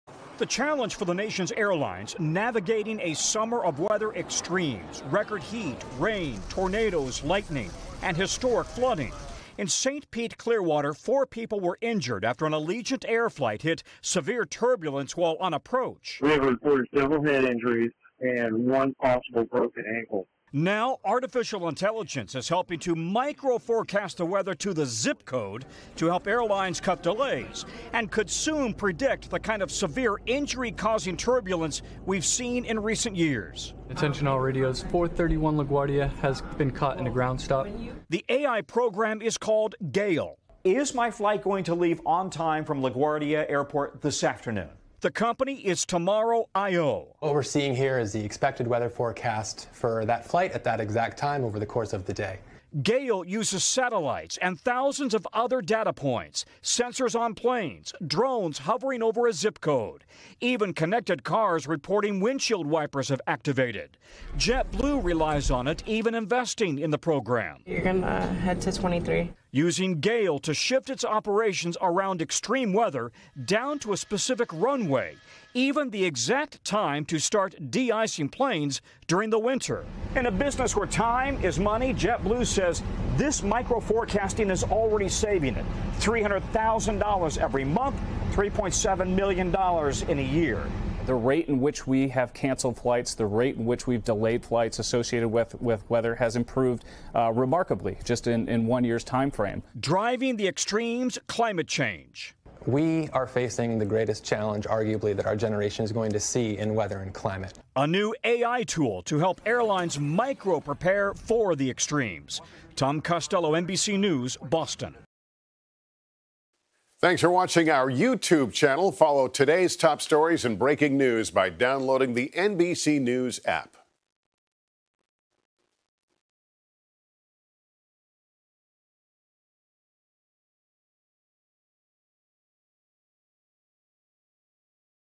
AI helping airlines forecast weather to reduce flight delays
The nation’s airlines are navigating a summer of weather extremes including record heat, rain, tornadoes, lightning and historic flooding. NBC News’ Tom Costello has more details on how artificial intelligence is now helping to micro-forecast the weather to help cut flight delays.